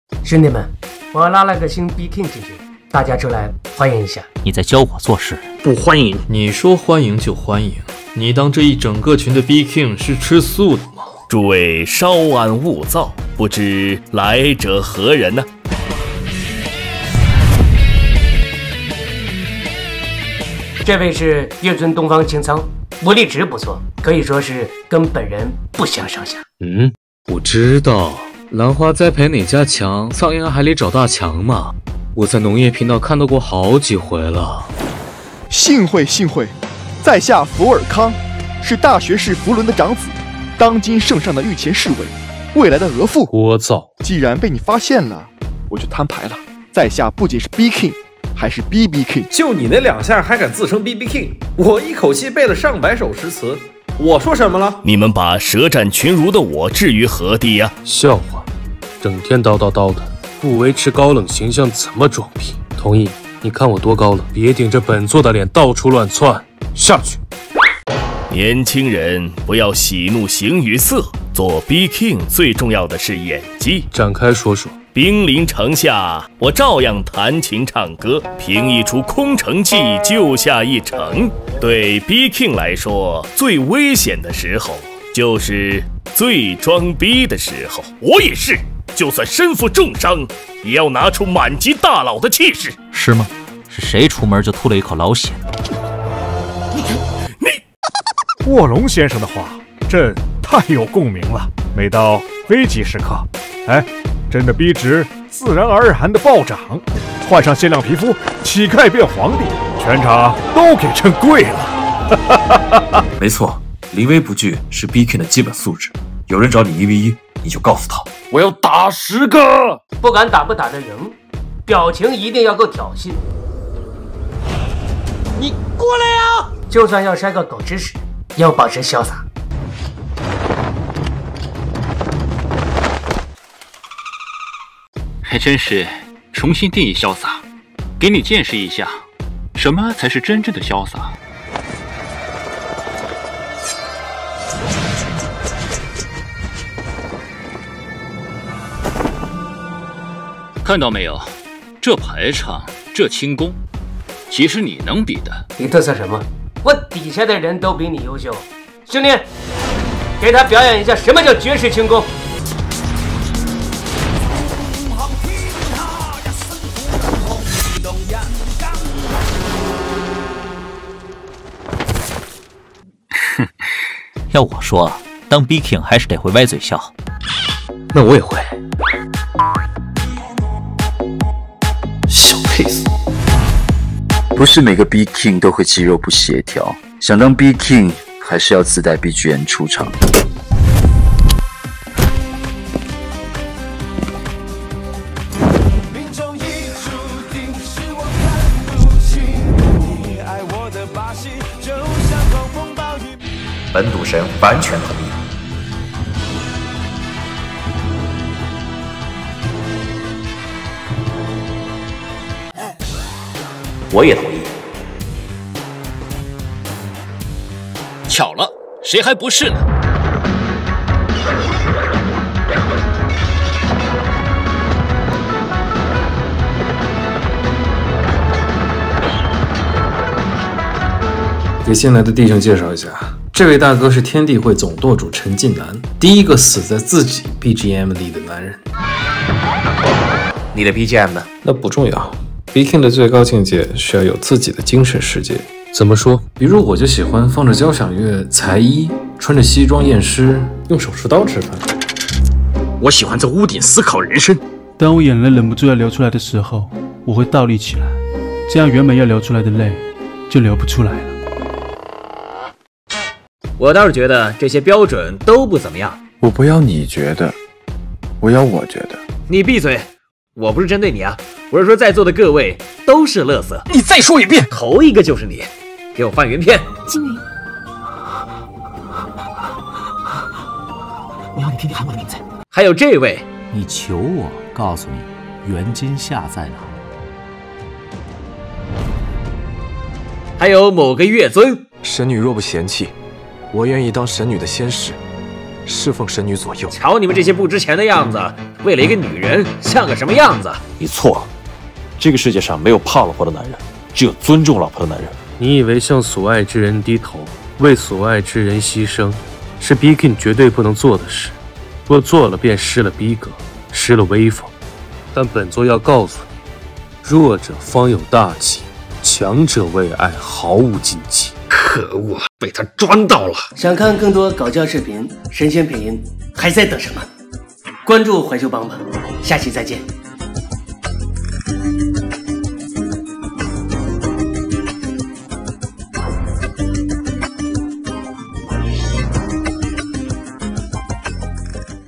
【淮秀帮】假如影视剧Bking在一个群！
副掌门邀请新晋Bking东方青苍加入群聊，陆绎、步惊云、羽皇、尔康、单均昊、秦明……谁能让大家心服口服？临危不惧、歪嘴笑、自带BGM……什么是Bking必备技能？可恶，被他们装到了！